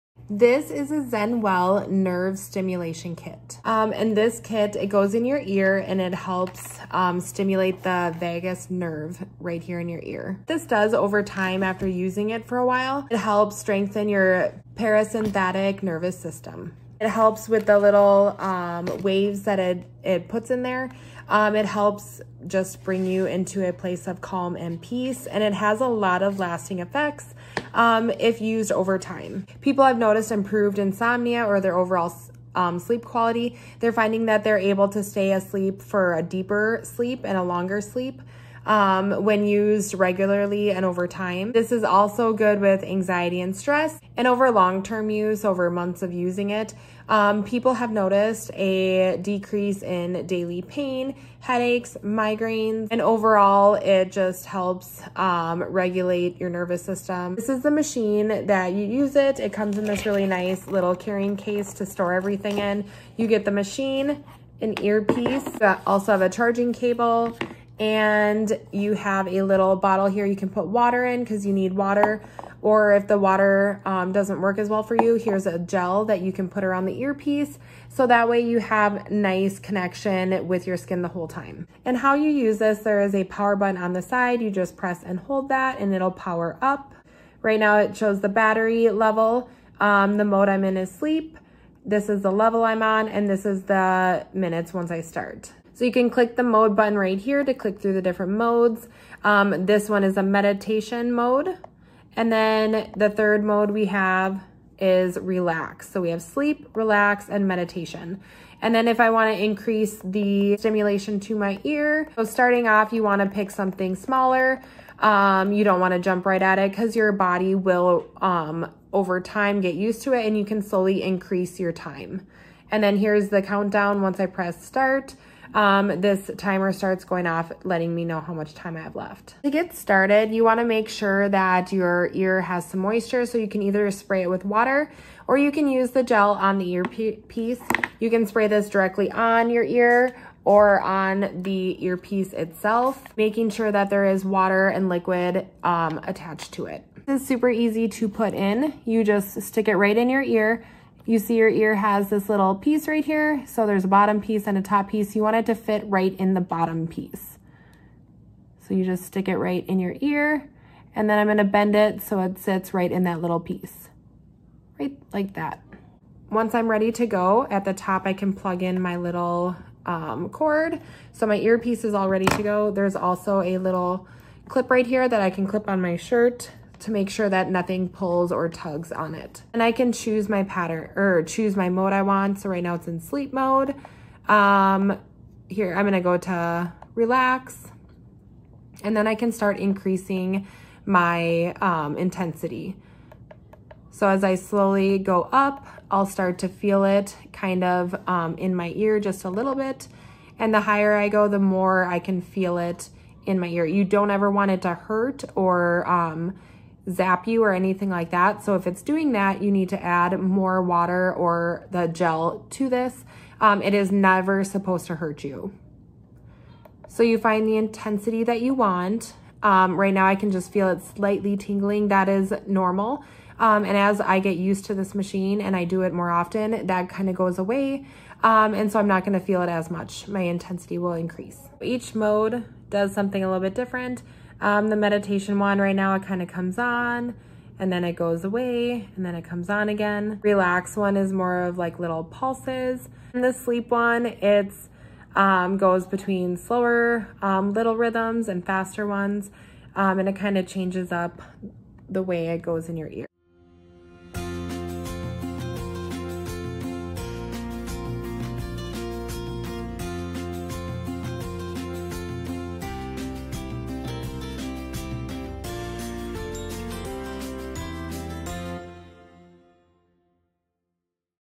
non-invasive vagus nerve stimulation from ZenoWELL
Following correct procedures to put on the earpiece is super important for the precision modulation of the auricular vagus nerve !!!